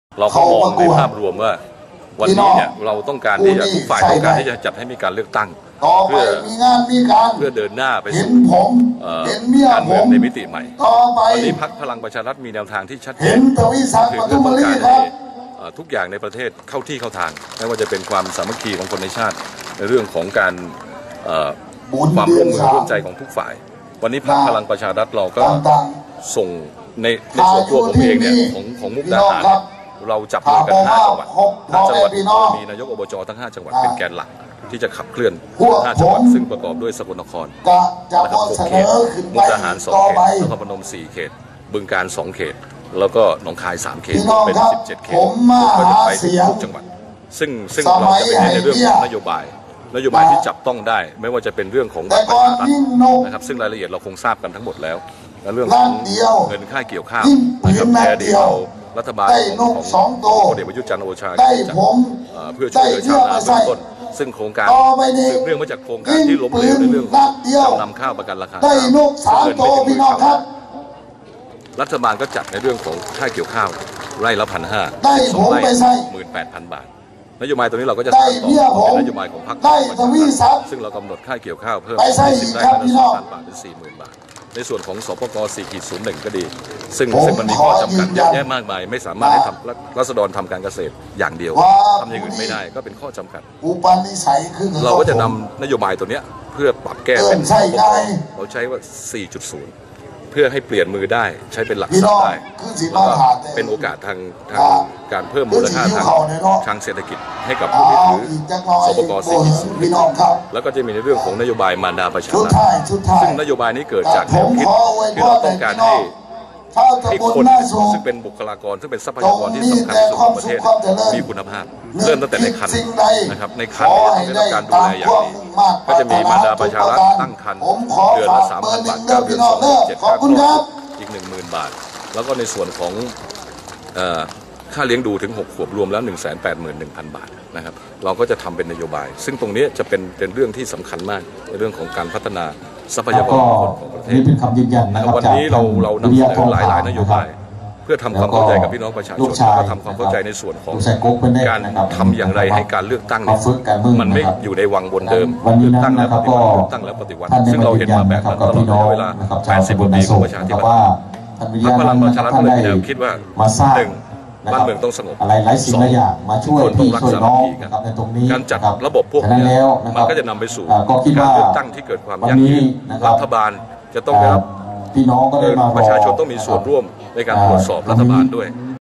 พลังประชารัฐระดมช่วยผู้สมัคร ส.ส. หาเสียง
นายกฤชณพล วิริยะบุญญา ผู้สมัครบัญชีรายชื่อจังหวัดสกลนคร ลงพื้นที่จังหวัดมุกดาหาร เปิดเวทีหาเสียงช่วยผู้สมัคร ส.ส.เขต 2 เบอร์ 1 ชูนโยบายบัตรสวัสดิการแห่งรัฐ มารดาประชารัฐ และพักชำระหนี้เกษตรกร
          เมื่อคืนวันที่ 6 มี.ค.62 ที่บริเวณหน้าเทศบาลตำบลนาโสก ตำบลนาโสก อำเภอเมือง จังหวัดมุกดาหาร นายกฤชณพล วิริยะบุญญา ผู้สมัครบัญชีรายชื่อจังหวัดสกลนคร ลงพื้นที่จังหวัดมุกดาหาร เปิดเวทีหาเสียงช่วยผู้สมัคร ส.ส.เขต 2 นายทวีศักดิ์ ประทุมลี เบอร์ 1 พรรคพลังประชารัฐ ชูนโยบายบัตรสวัสดิการแห่งรัฐ มารดาประชารัฐ และพักชำระหนี้เกษตรกร ให้กับชาวตำบลนาโสก อำเภอเมือง จังหวัดมุกดาหารจำนวน 16 หมู่บ้าน กว่า 1,000 คน มาให้กำลังใจคณะผู้สมัคร ส.ส. เขต 2
          นายกฤชณพล วิริยะบุญญา ผู้สมัครบัญชีรายชื่อจังหวัดสกลนคร กล่าวว่า ในวันนี้มีโอกาสมาช่วยผู้สมัคร ส.ส. เขตเลือกตั้งที่ 2 จังหวัดมุกดาหาร นายทวีศักดิ์ ประทุมลี ในนามผู้สมัคร ส.ส.บัญชีรายชื่อพรรคพลังประชารัฐ อยากให้มีการเลือกตั้ง เพื่อเดินหน้าไปสู่การเมืองในมิติใหม่ พรรคมีแนวทางที่ชัดเจน อยากให้ทุกอย่างในประเทศเข้าที่เข้าทาง ไม่ว่าจะเป็นความสามัคคีของคนในชาติ และความร่วมมือร่วมใจของทุกฝ่าย พรรคพลังประชารัฐภาคอีสานได้จับมือกัน 5 จังหวัด ประกอบด้วย สกลนคร 6 เขต มุกดาหาร 2 เขต นครพนม 4 เขต บึงกาฬ 2 เขต และหนองคาย 3 เขต รวม 17 เขต
          นายกฤชณพล กล่าวเพิ่มเติมว่า ซึ่งจะเน้นในเรื่องนโยบายที่จับต้องได้ไม่ว่าจะเป็นนโยบายของพรรคพลังประชารัฐ ชูนโยบายบัตรสวัสดิการแห่งรัฐหรือบัตรประชารัฐ ซึ่งจะมีการเพิ่มคน เพิ่มสิทธิให้กับประชาชนที่ตกหล่น นโยบายพักหนี้กองทุนหมู่บ้าน 3 ปี โดยการพักชำระหนี้ ฟื้นฟู เติมทุน และให้โอกาส นโยบาย ส.ป.ก.4.0 ใช้ประโยชน์ เพิ่มมูลค่า นโยบายเกษตรประชารัฐ 4.0 การแก้ปัญหาราคาข้าวให้เพิ่มสูงขึ้น ชดเชยช่วยเหลือชาวนาค่าเก็บเกี่ยวจากไร่ละ 1,500 บาท เป็นไร่ละ 2,000 บาท และเพิ่มพื้นที่จากเดิมรายละไม่เกิน 12 ไร่ เป็น 20 ไร่ รวมทั้งนโยบายมารดาประชารัฐถ้วนหน้า โดยการดูแลตั้งแต่ตั้งครรภ์ เดือนละ 3,000 บาท เป็นเวลา 9 เดือน รวม 27,000 บาท และค่าคลอดอีก 10,000 บาท และได้ทำความเข้าใจกับประชาชนทำอย่างไรให้การเลือกตั้งไม่อยู่ในวังวนเดิม พรรคพลังประชารัฐมีแนวคิดว่าบ้านเมืองต้องสงบ คนในชาติต้องสามัคคีกัน การเลือกตั้งก็จะนำไปสู่ความยั่งยืน ประชาชนต้องมส่วนร่วมในการตรวจสอบ ฝากพี่น้องประชาชนทั้งประเทศต้องมาช่วยกัน หันหน้าเข้าหากัน จับมือกันรวมใจกันให้เป็นหนึ่ง เพื่อก้าวข้ามปัญหาอุปสรรคข้อจำกัดต่าง ๆ ของการเมืองไทย อยากให้ประชาชนได้ประโยชน์สูงสุด นึกถึงประชาชน นึกถึงประโยชน์ประเทศชาติเป็นหลัก ซึ่งได้รับความสนใจจากประชาชน
          อย่างไรก็ตาม นายทวีศักดิ์ ประทุมลี ย้ำว่าหากตนได้รับการเลือกตั้งเป็น ส.ส.จังหวัดมุกดาหาร เขต 2 ในนามพรรคพลังประชารัฐ จะเข้าสานงานต่อนโยบายของรัฐบาลต่อไป./อนุศักดิ์ – เสาวภา แสนวิเศษ / รายงาน